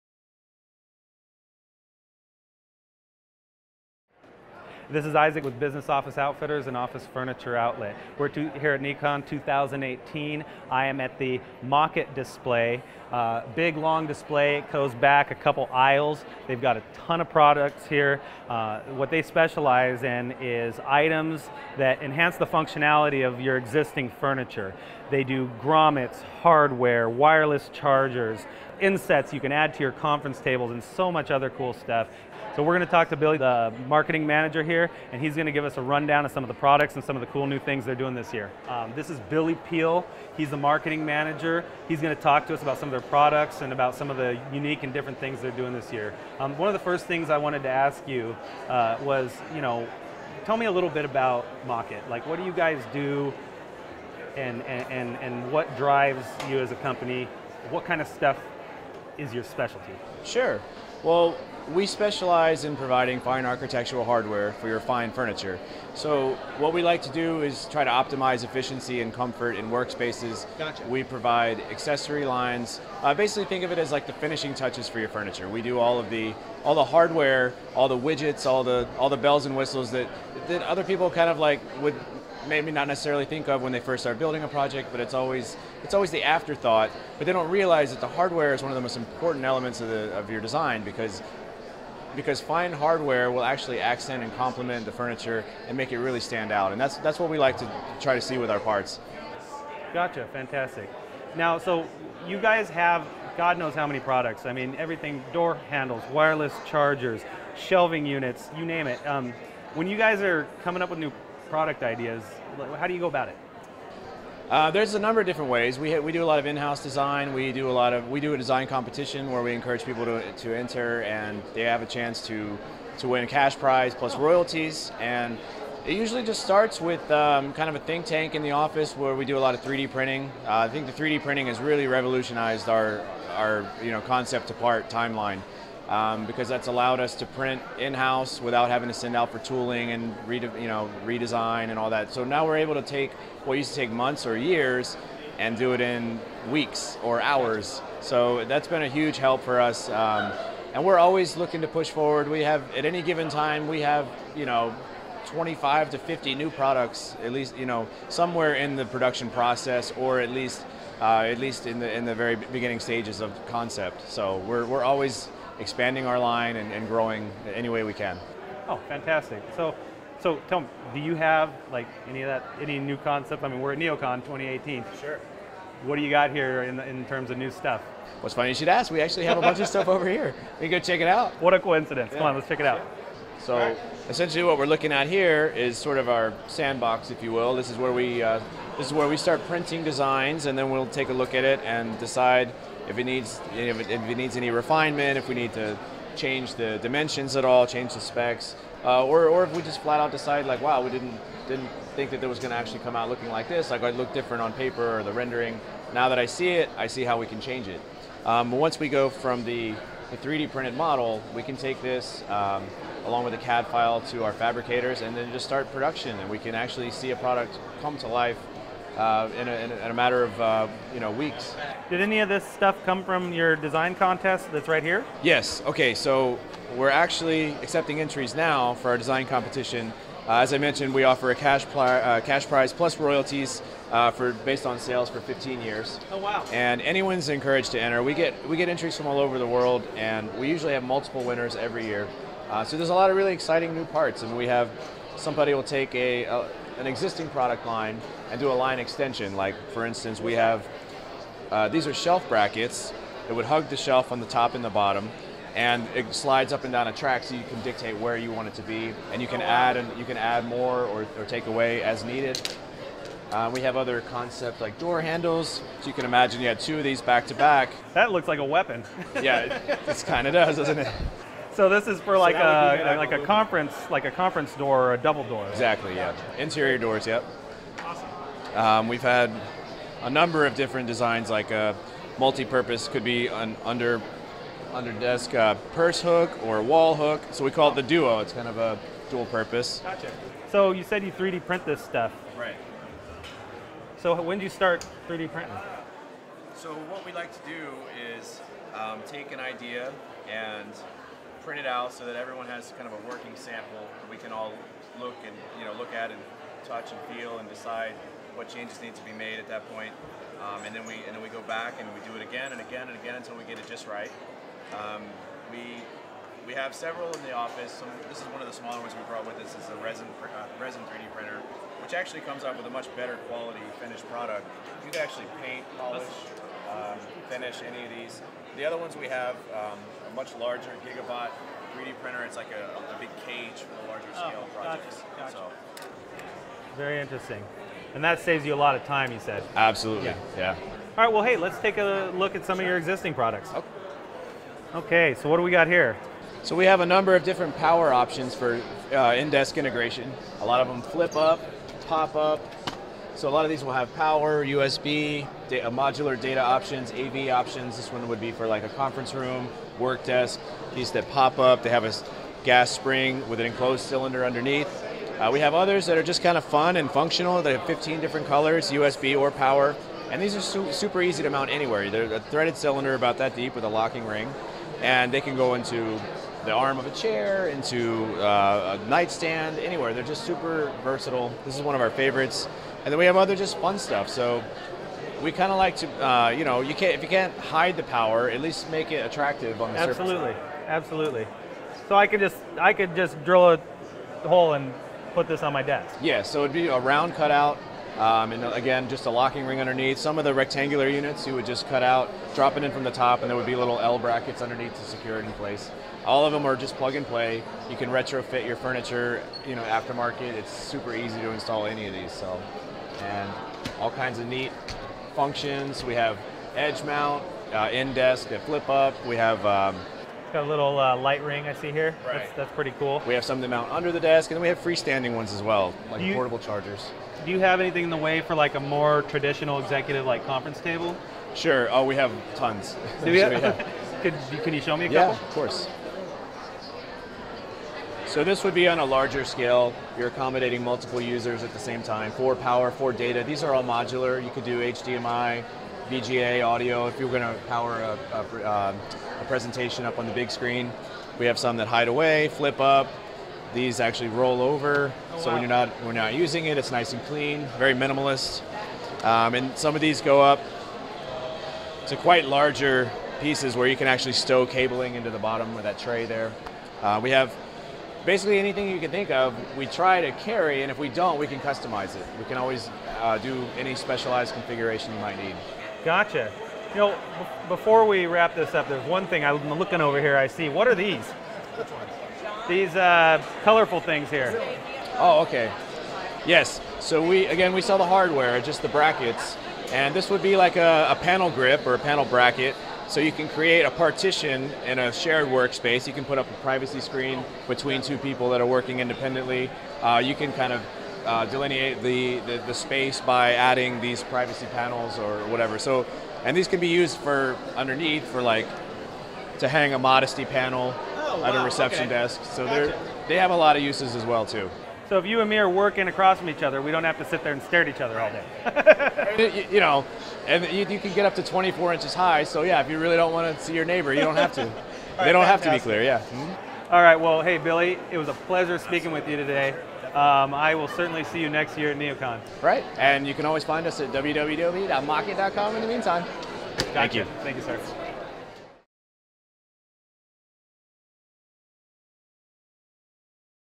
Best of Neocon - Mockett - Conference Room Table Power Outlets
Conference Room Table Power Outlets

Say hello to Billy Peele with Mockett. Mockett specializes in providing fine architectural hardware for your fine furniture to optimize efficiency and comfort in your workplace.

They provide accessory lines like power grommets, wire management, drawer pulls, knobs & door handles, furniture legs & casters, desk accessories, Shelf brackets, standoffs & signage, widgets, hardware, wireless chargers, in-sets you can add to your conference tables and so much other cool finishing touches.

What makes Mockett so cool is that they understand that hardware is one of the most important elements of the office space design and they build and design hardware that accents and compliments the furniture.